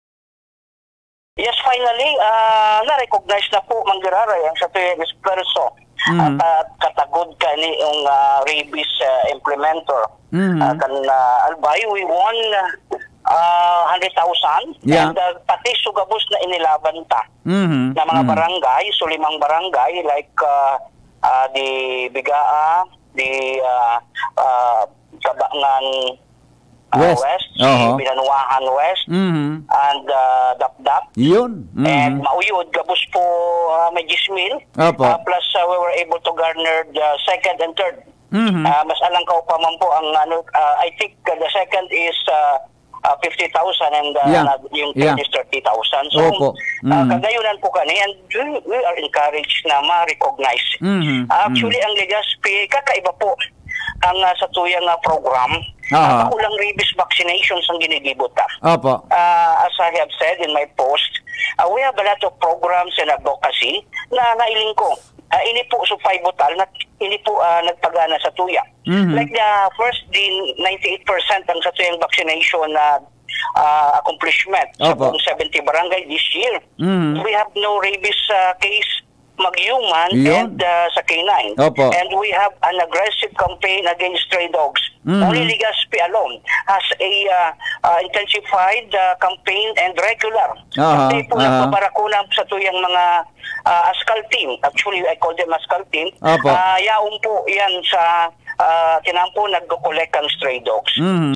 𝐋𝐄𝐆𝐀𝐙𝐏𝐈 𝐂𝐈𝐓𝐘 𝐕𝐄𝐓 𝐎𝐅𝐅𝐈𝐂𝐄 𝐍𝐀𝐌𝐄𝐃 𝐀𝐒 𝐀𝐋𝐁𝐀𝐘’𝐒 𝐁𝐄𝐒𝐓 𝐈𝐌𝐏𝐋𝐄𝐌𝐄𝐍𝐓𝐎𝐑 𝐎𝐅 𝐀𝐍𝐓𝐈-𝐑𝐀𝐁𝐈𝐄𝐒 𝐏𝐑𝐎𝐆𝐑𝐀𝐌
Dakula an pasasalamat ni Legazpi City Veterinary Officer Dr. Manny Estipona matapos na pangaranan an syudad nin Legazpi bilang Best Implementor kan Anti-Rabies Program sa syudad nin Legazpi.

Ini an sinabi ni Estipona sa intrivista kan DZGB News kun sain saiyang ipinaliwanag na maogmang marhay an saindang opisina asin mga personahes dahil sa matapos an mahiwasang eswpersos para mapakarhay an siring na programa, pormal sindang natawan nin rekonisimiento kan Albay Provincial Veterinary Office.

Tune in for more updates.